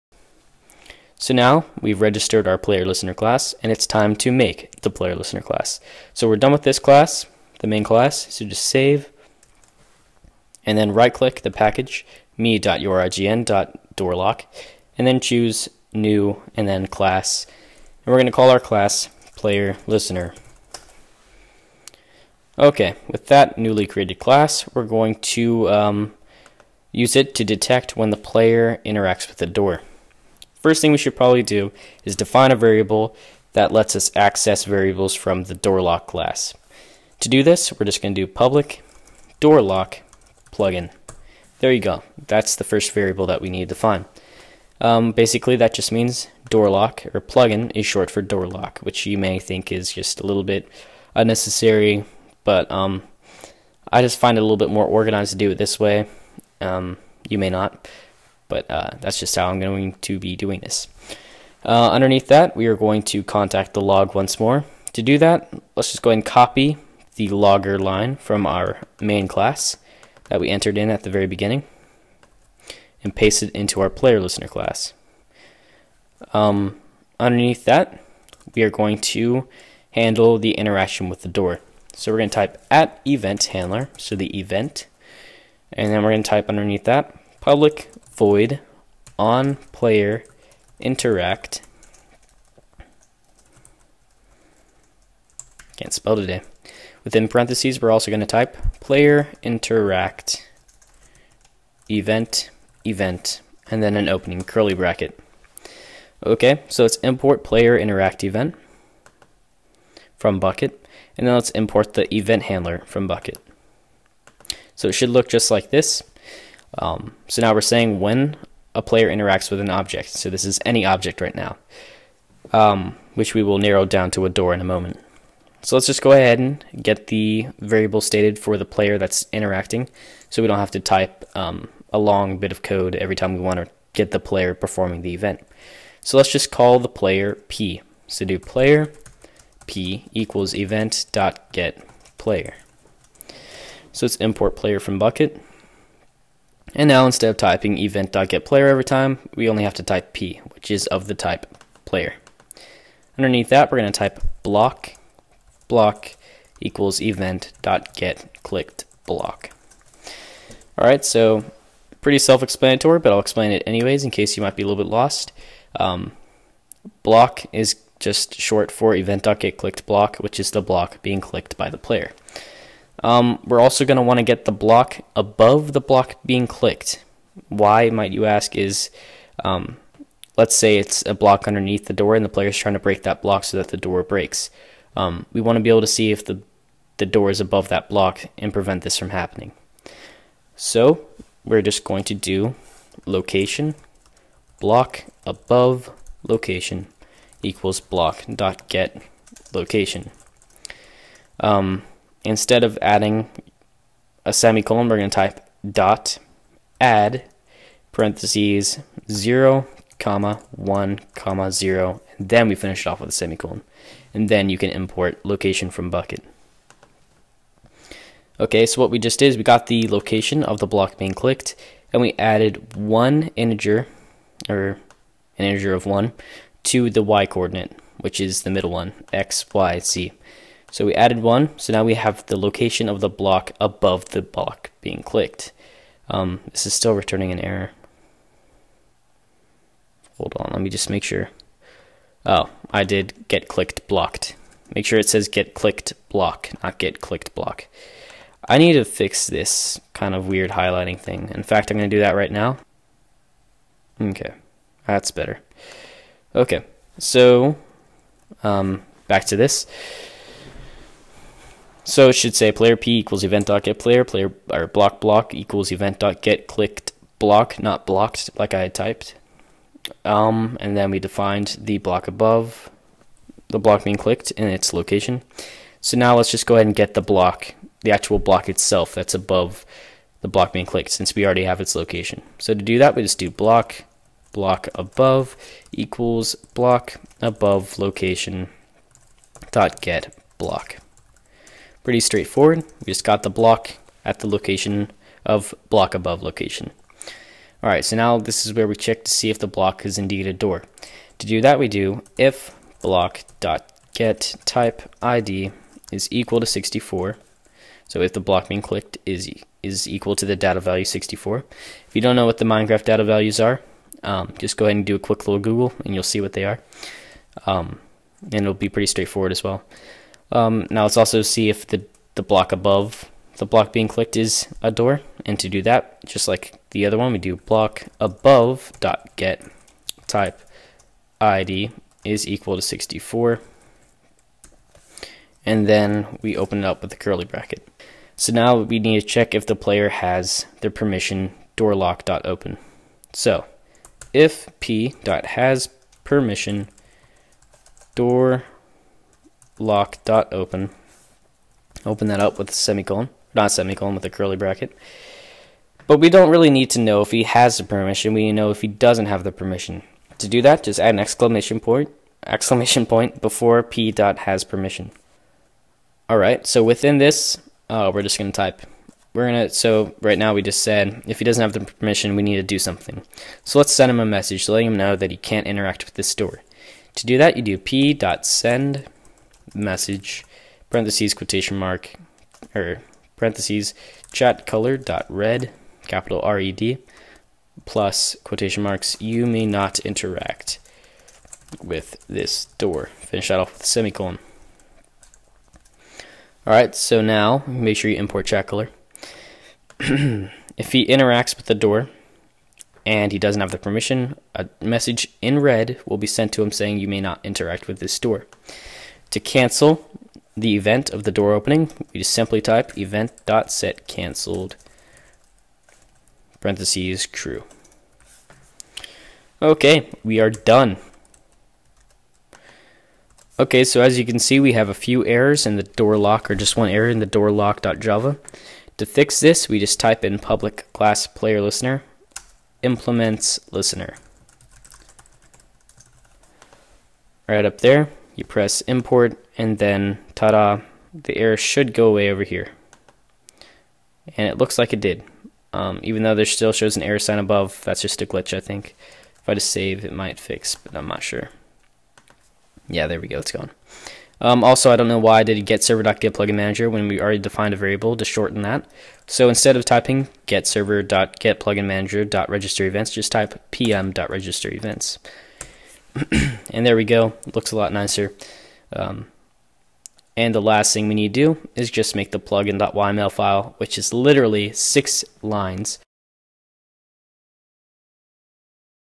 Episode 2: Door Protection Bukkit Plugin Tutorial
Door Protection Bukkit Plugin Tutorial - Episode 2

Learn how to make a plugin that locks doors from players with a certain permission.

In this episode, we create and finish our Player Listener class to prevent players without the certain permission from opening doors.

** IMPORTANT UPDATE TO CODE **

Please add the following line right underneath the "onPlayerInteract" line:

If(event.getAction()==Action.RIGHT_CLICK_BLOCK || event.getAction()==Action.LEFT_CLICK_BLOCK) {

Right above the closing curly bracket ( } ) for the "onPlayerInteract" method, please place another closing curly bracket: }

Credit to 'nicka1011' for code correction.